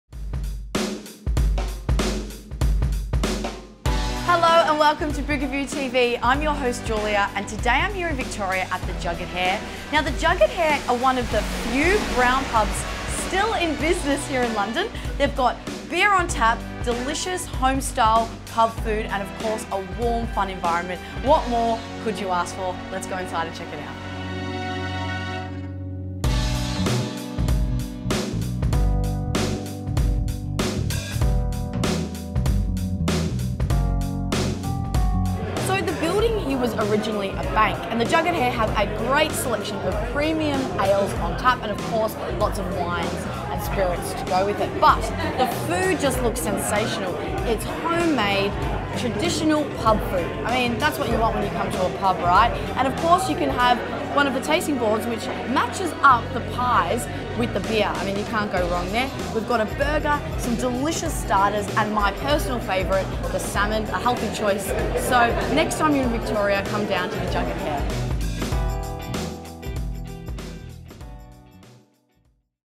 The Jugged Hare, a Gastropub in London serving Sunday Roast, Pub Food, and Beer
The Jugged Hare is a gastro pub & restaurant in the City of London, famous for their Sunday roasts and a specialism in the British game, along with spit-roast meats and wild fish.

The Jugged Hare
172 Vauxhall Bridge Rd, Westminster London UK SW1V 1DX